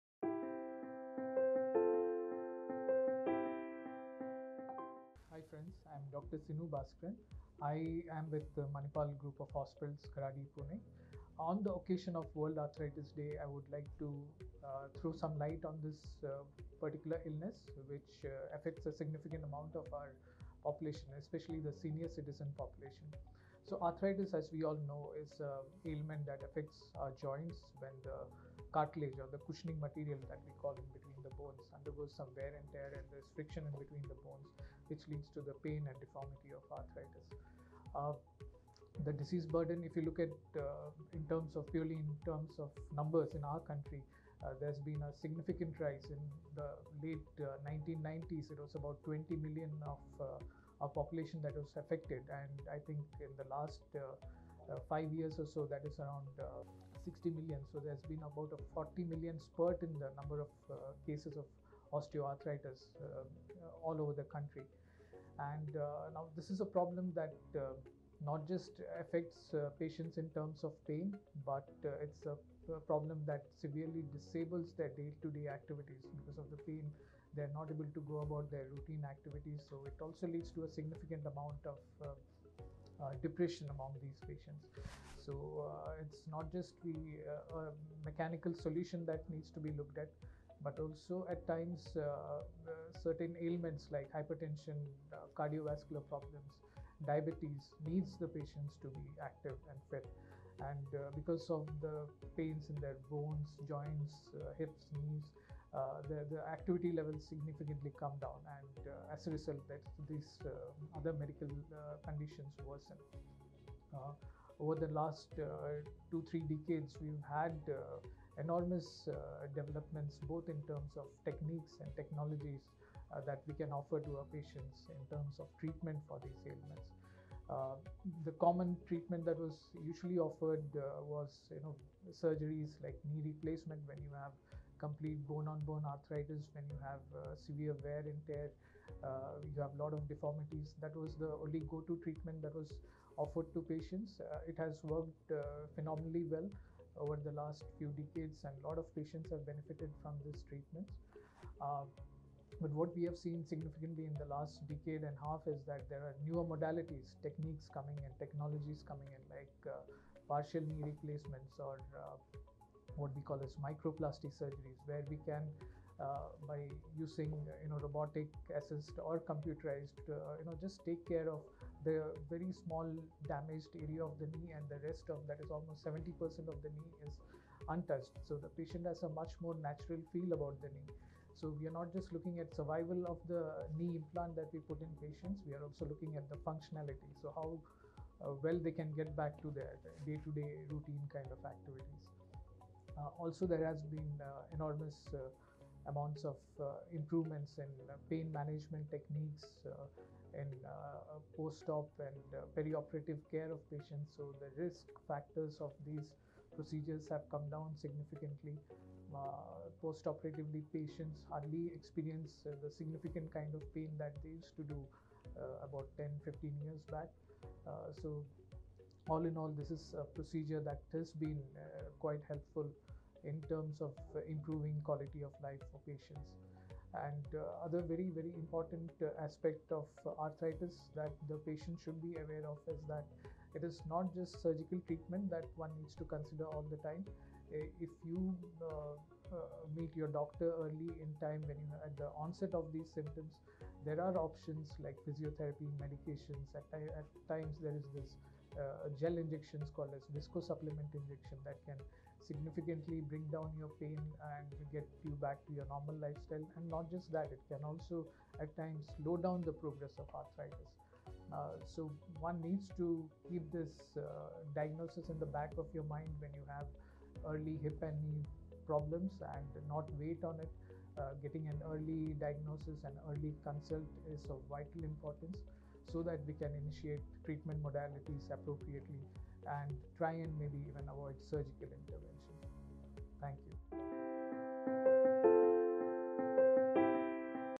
World Arthritis Day | Dr. Sinukumar Bhaskaran | Manipal Hospital Kharadi
On World Arthritis Day, dive deep into arthritis insights with Dr. Sinukumar Bhaskaran, Consultant - Orthopedics & Joint Replacement Surgeon at Manipal Hospital Kharadi, Pune. Watch the video to learn about the advanced treatments available at the hospital for effective recovery.